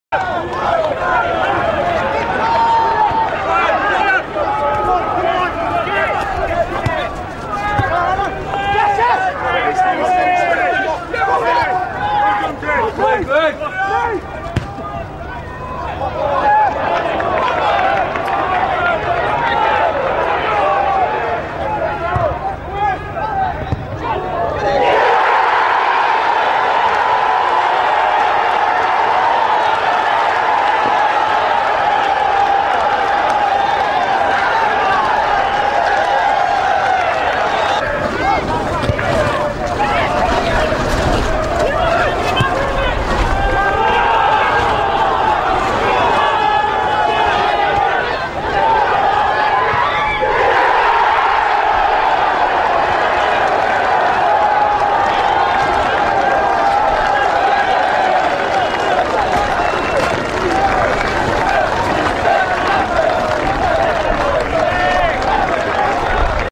Alfreton Town 0-2 Harriers 21/01/12: The goals
Anthony Malbon nets on his Harriers debut as the team pick up a great 2-0 win at Alfreton.